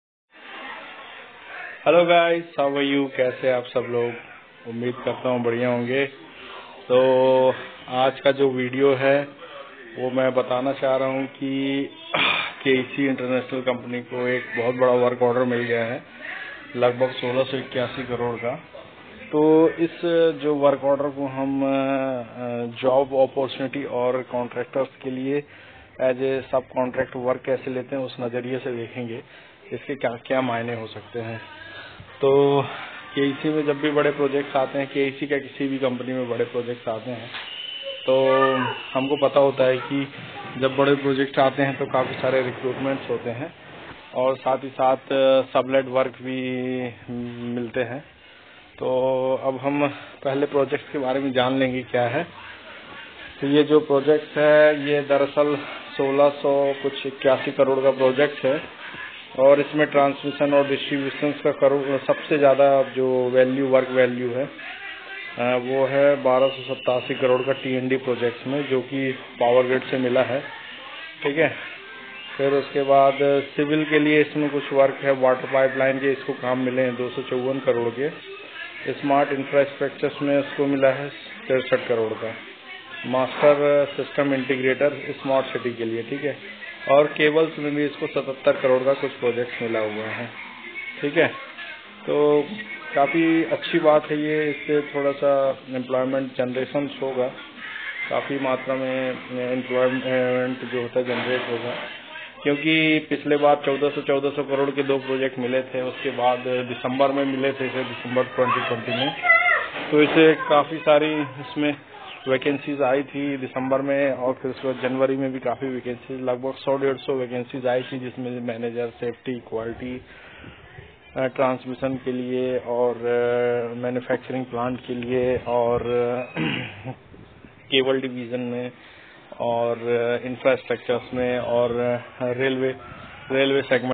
KEC International Limited for 1681 karod ka project Mila. job opportunity
kec upcoming projects